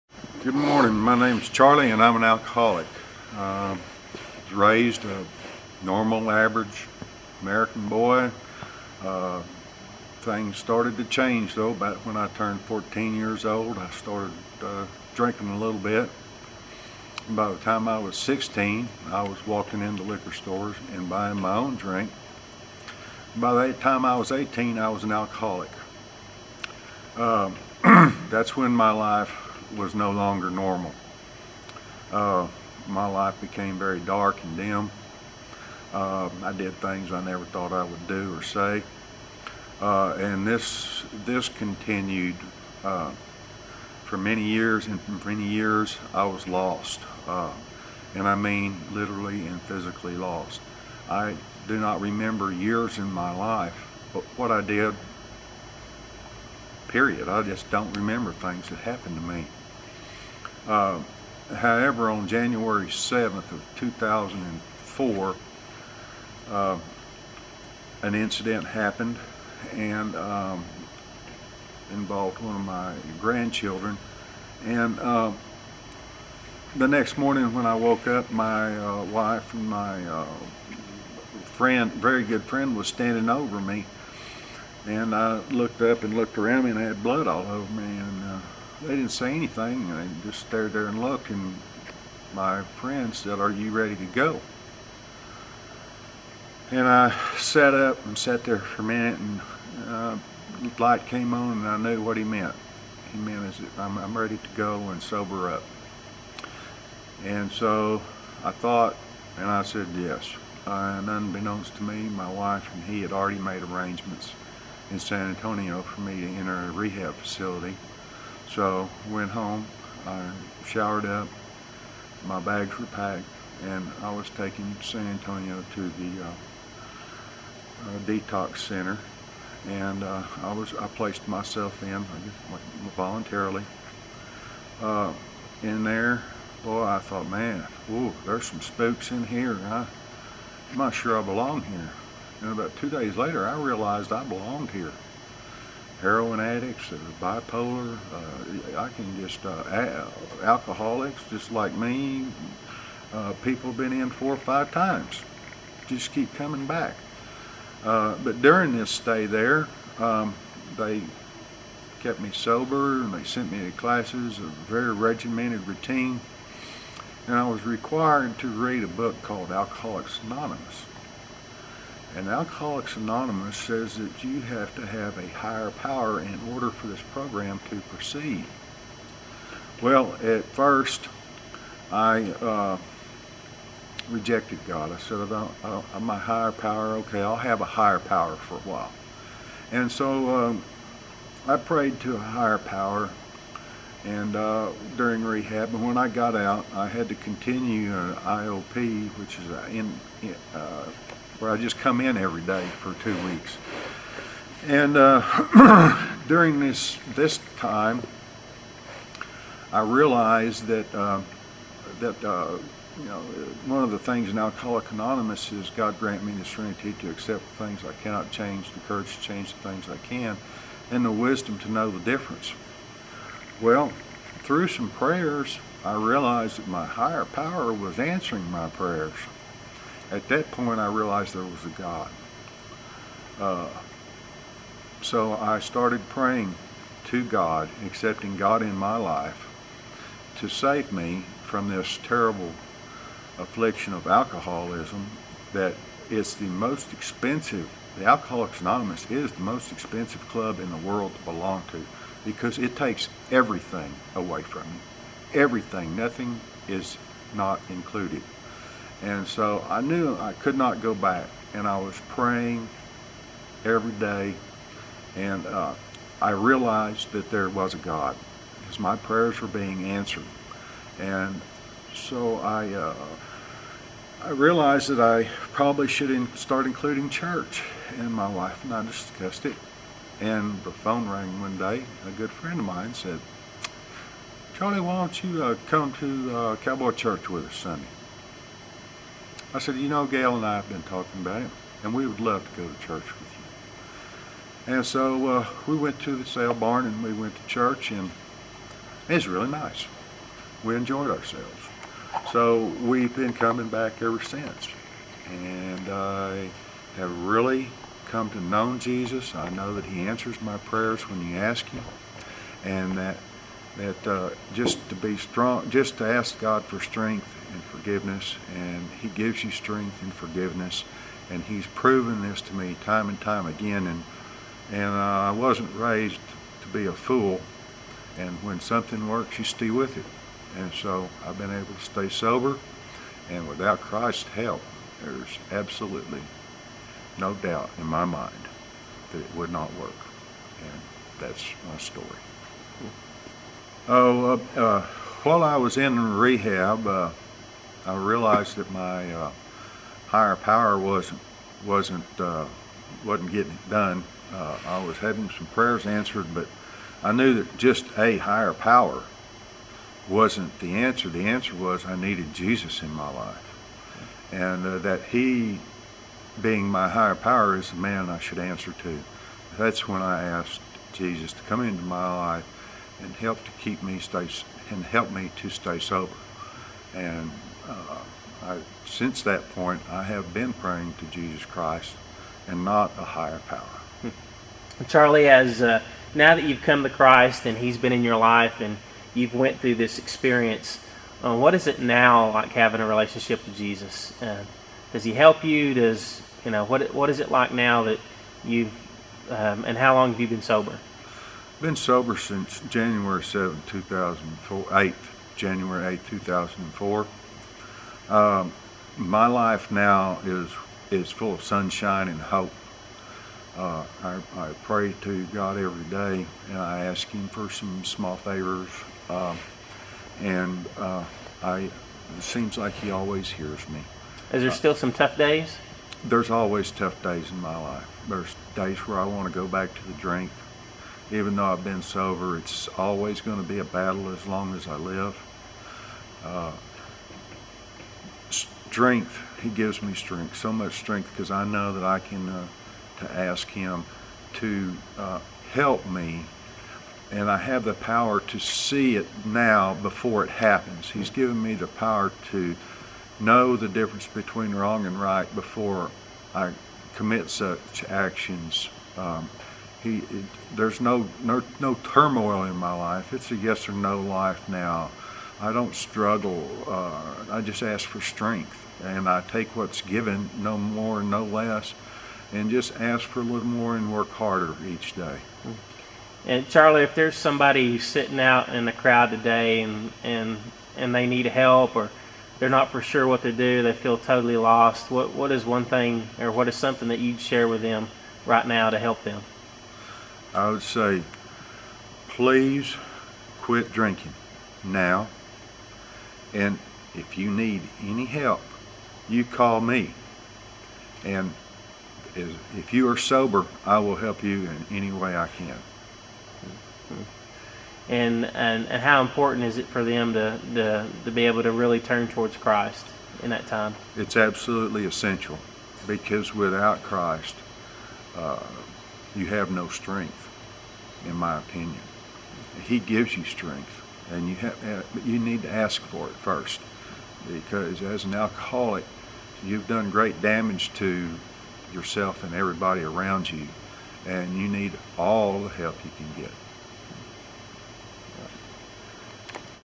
Uncut Charlie Walters
Testimony of a saved Man who wants others to be able to discover the life that Jesus has to offer as well as the help & strength.